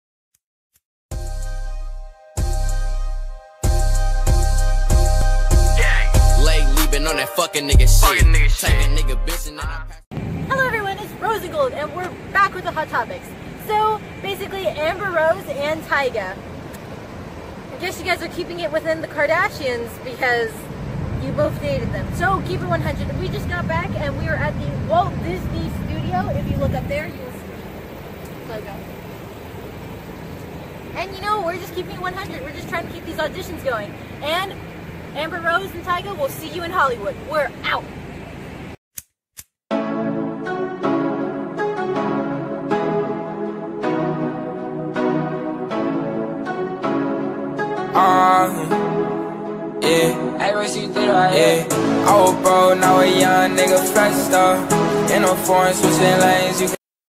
WALT DISNEY ROSENGOLD
Two young firecrackers ready to take Talk Show Scene by the storm, move over Wendy Williams, there is a new group in town, we would like to have Wendy William as our guest, How you doin!

 Hi guys! today we did another video of the show, Rose and Gold! Watch our latest video to see what todays hot topics are!